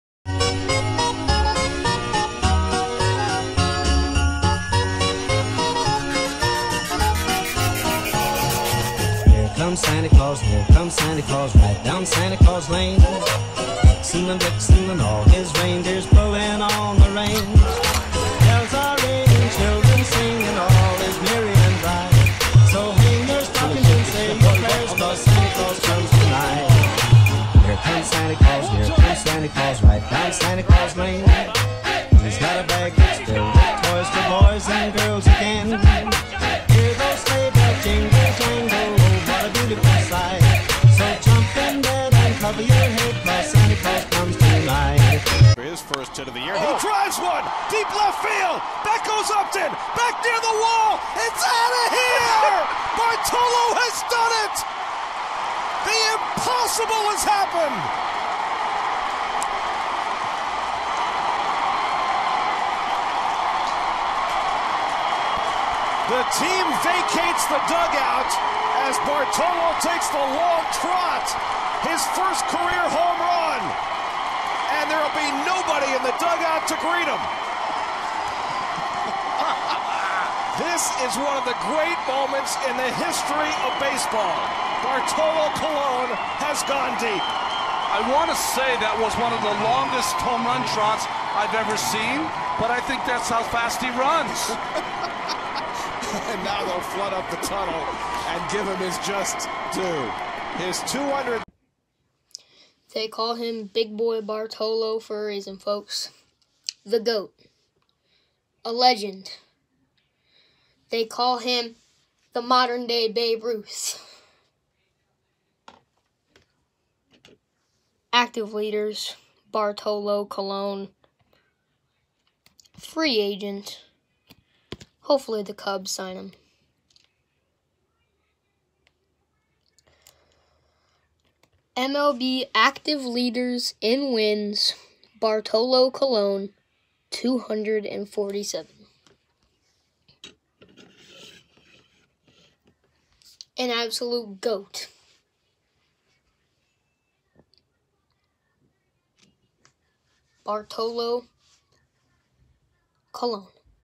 Bartolo Colon 🐐 Cards & Baseball Talk | 18 Days Til Christmas!! CardCollector Christmas Ep. 8
I hate goin on everyone!!

In today’s video, I will be talking about The GOAT, Big Sexy, Big Boy Bartolo, Babe Ruth’s Great Grand Son. Merry Christmas!!

I hope you all enjoyed and I’ll catch you later!!🎄✌️